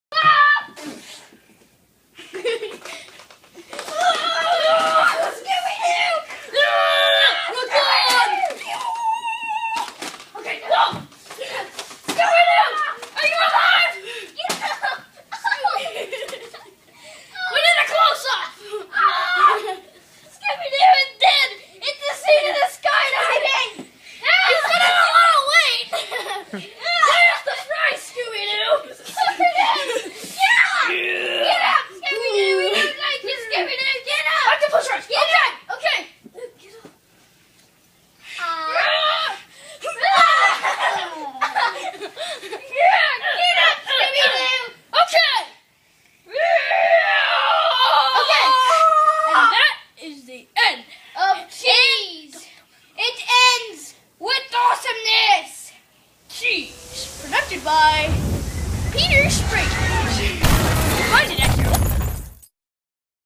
Cheese 2 Night of the Unliving Scooby-Doo
It is an awesome video.Scooby-Doo(Luke Sweat),Kid with glasses(Andrew Sweat),& kid with hat(Nick Williams).